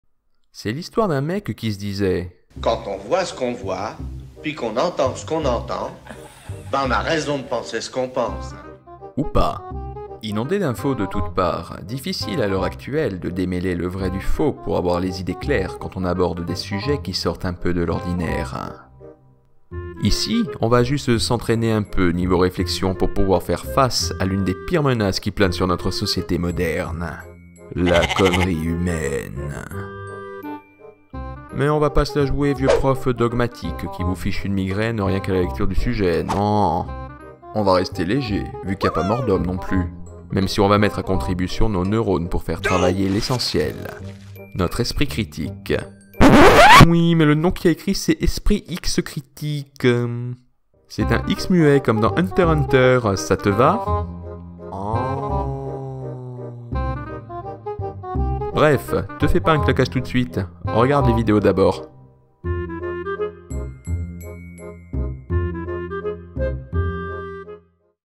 [ANNONCE] Esprit X Critique - C'est bon pour tes neurones
Juste une annonce quant à l'ouverture
 d'une seconde chaîne, destinée comme son nom le laisse supposer à affûter notre esprit critique ainsi que notre réflexion.

Je préfère séparer CLAIREMENT mon activité ici des sujets que je compte aborder sur Esprit X Critique, non pas que ceux-ci ne soient réellement polémique (pas question de grand clash des idées non plus...), mais un ton radicalement différent de la dominante calme et plutôt sereine que je compte garder ici.

Une première vidéo arrive TRÈS prochainement, où vous serez mis à contribution ;)

Musique